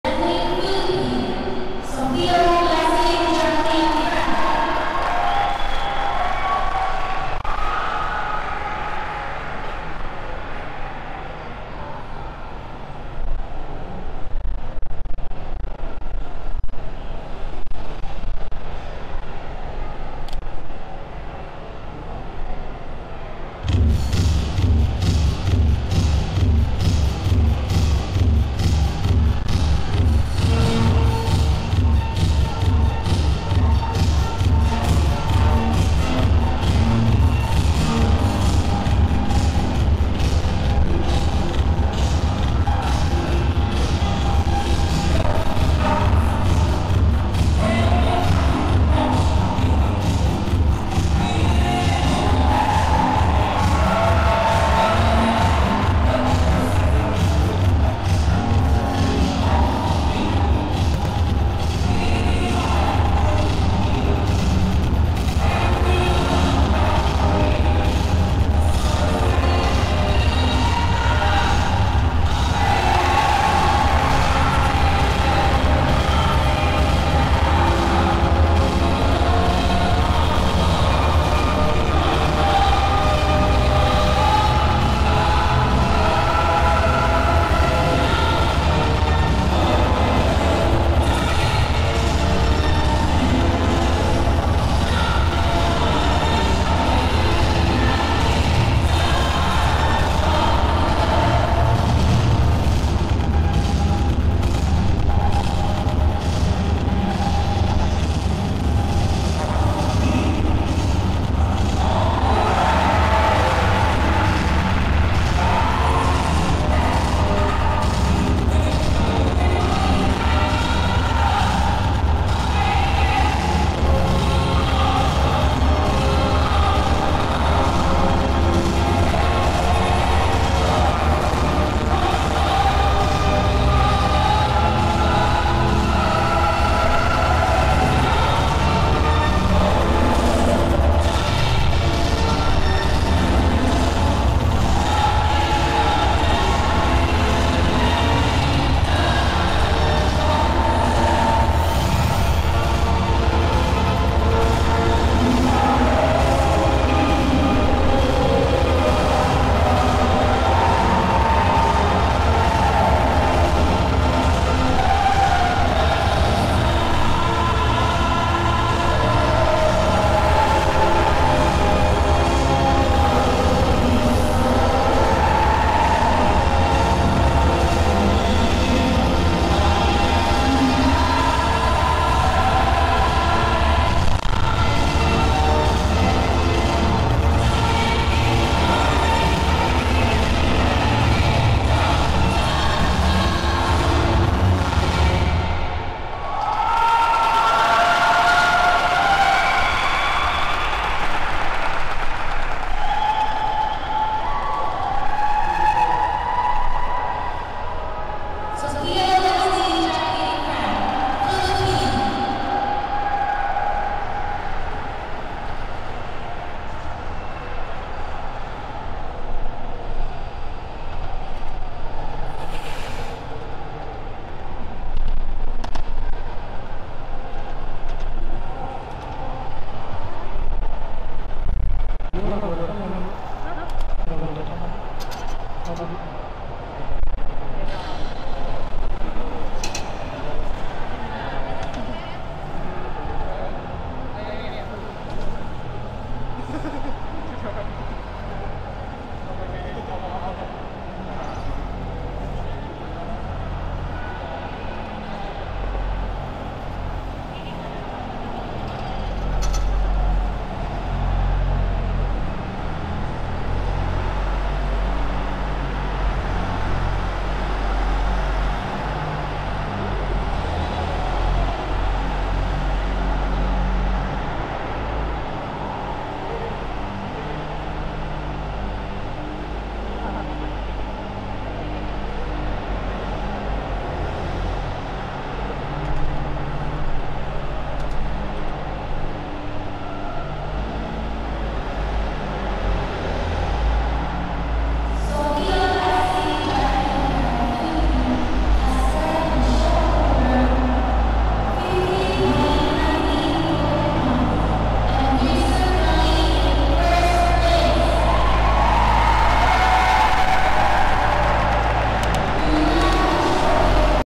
Asian Open Trophy 2022 - Sofia Frank 1st Place Short
1st Place Short Program Asian Open Figure Skating Trophy Jakarta Indonesia Dec 2022

Choreography:  Drew Meekins
Coach by Tammy Gambill
Philippine Skating Union